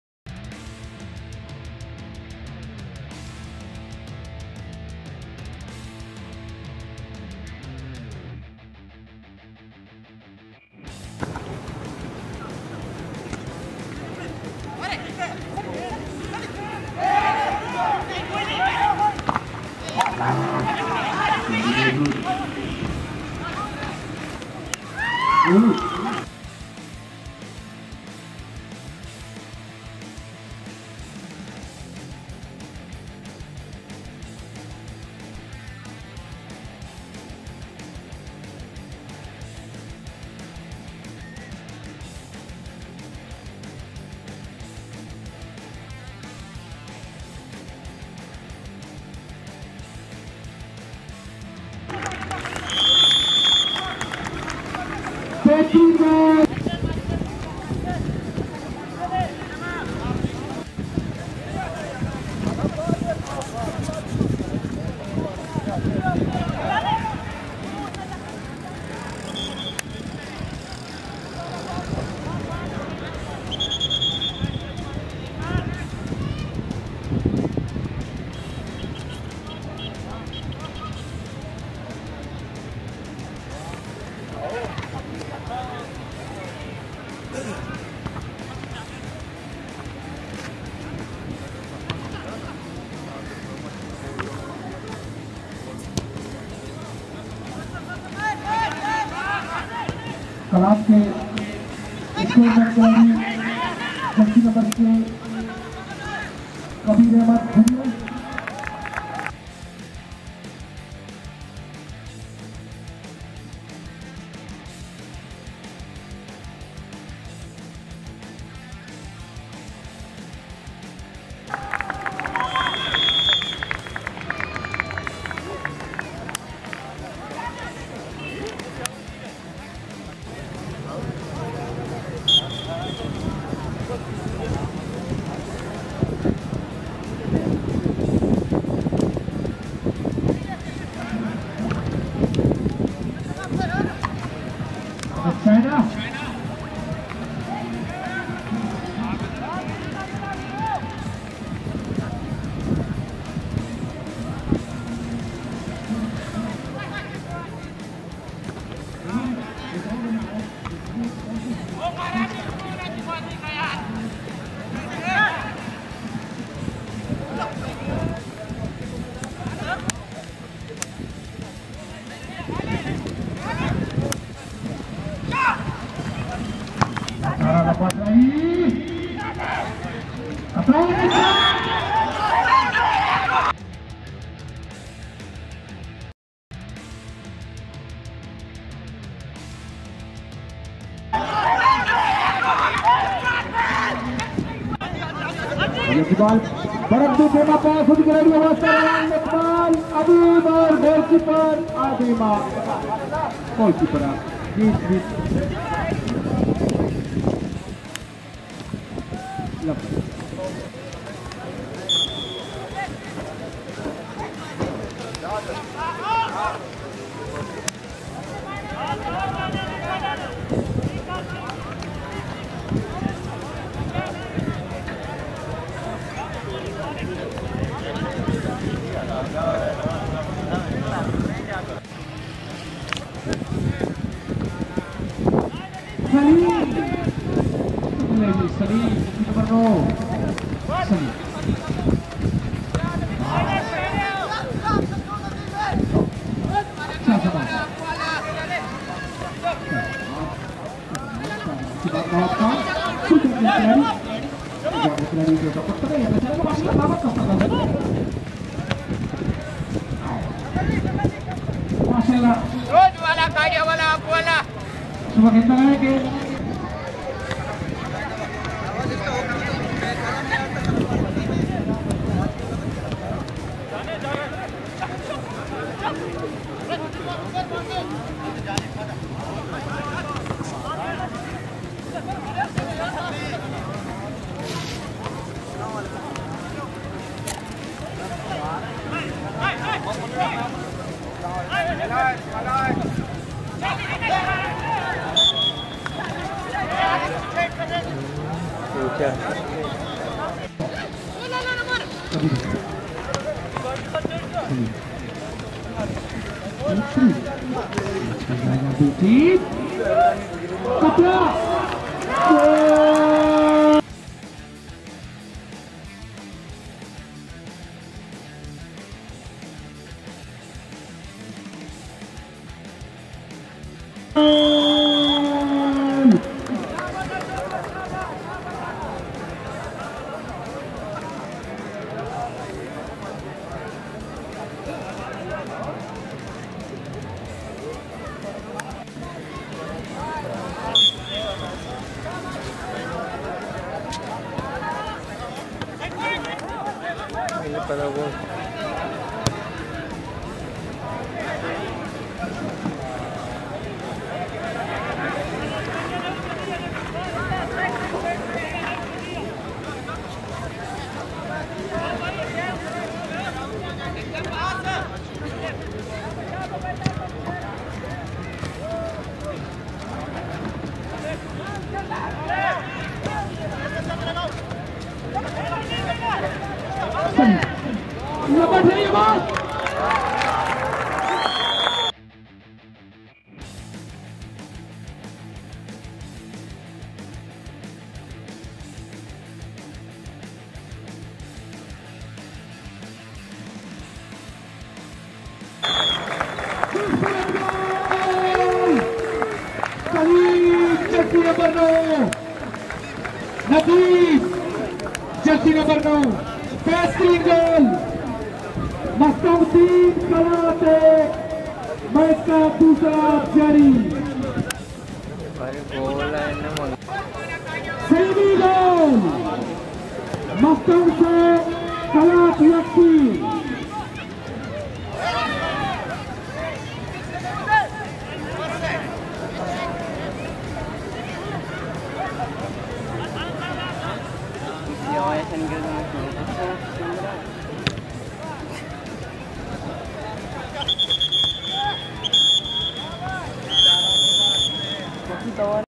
Khalat Vs Mastung 2nd day football match at Ibrahim Pele (Soragdil) Football stadium Gwadar
Khalath Vs Mastung Football match at Ibrahim Pele (Soragdil) Football stadium Gwadar | Meeran Baksh Memorial Sindh Balochistan football tournament 2021|Gwadar Football